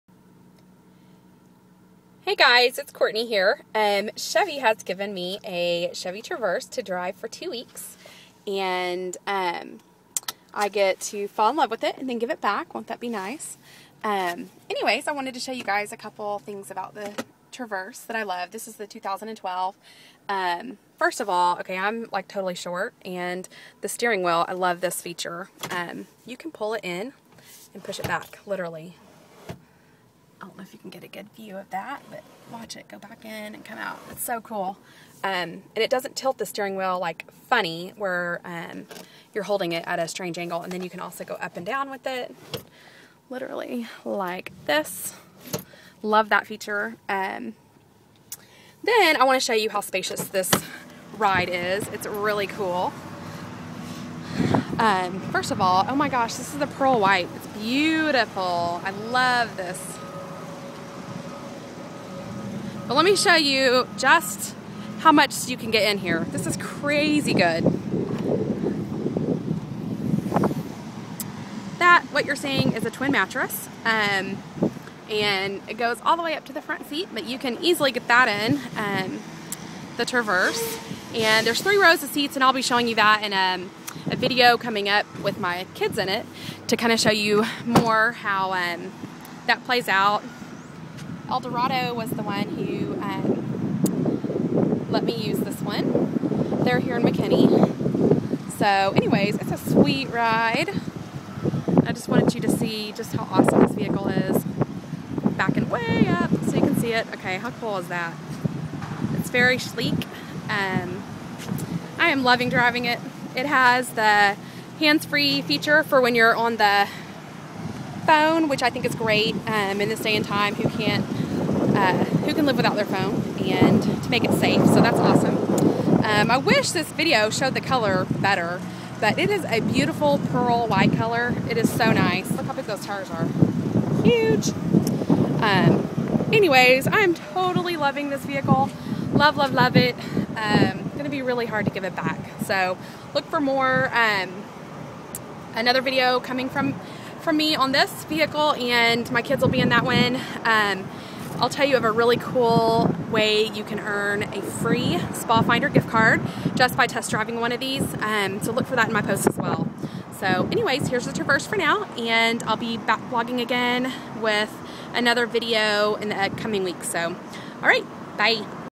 Loving the Chevy Traverse!
Chevy Girl tells about the 2012 Chevy Traverse!